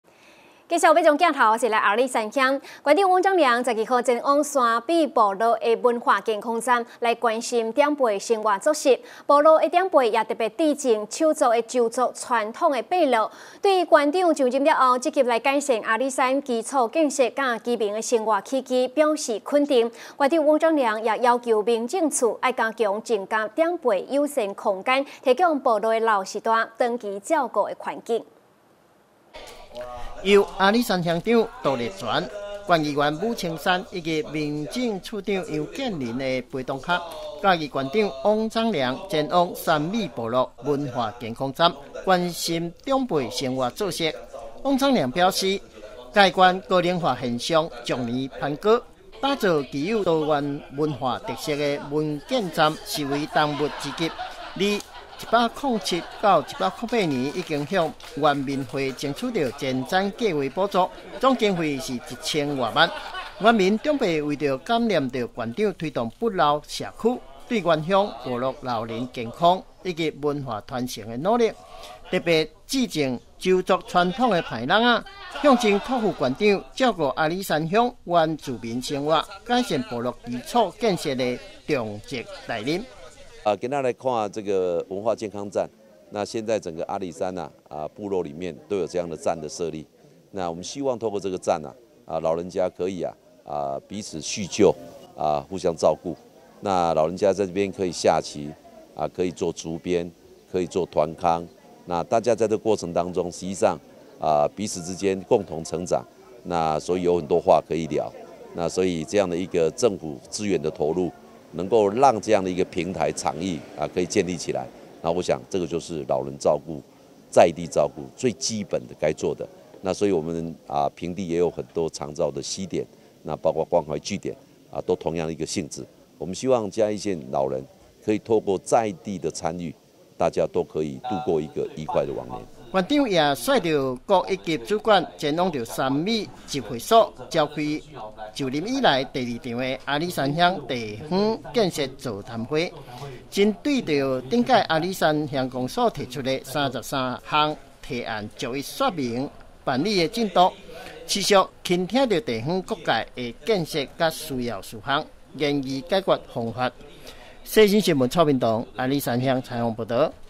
世新新聞 縣長視察山美部落文化健康站
108年7月15日 (一) 世新新聞

縣長視察山美部落文化健康站

縣長翁章梁12日前往山美部落文化健康站關心長輩生活作息，部落長輩也特別致贈手作鄒族傳統背簍，對縣長上任後積極改善阿里山基礎建設與居民生活起居，表示肯定，縣長翁章梁也要求民政處加強增加長輩友善空間，提供部落老勢大長期照顧的環境。